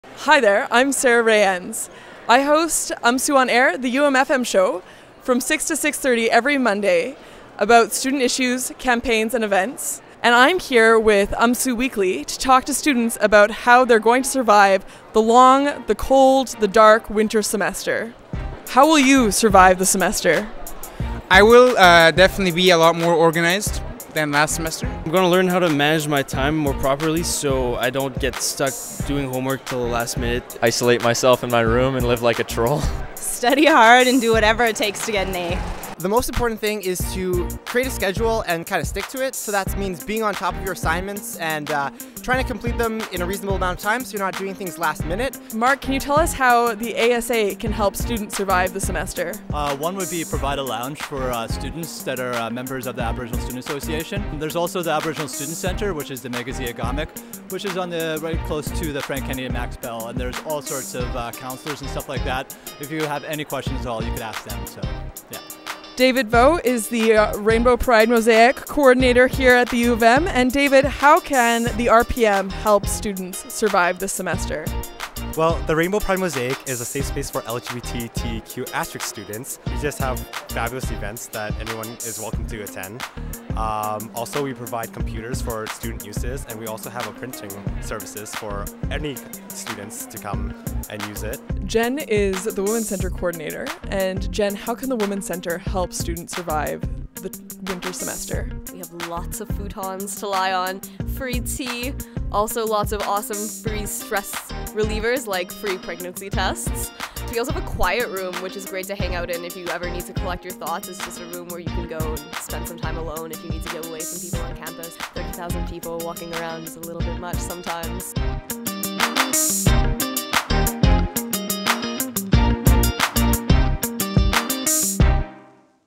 UMSU WEEKLY #12 - How to Survive the Winter Semester
Sara from UMSU On Air talks to students about how to survive the winter semester.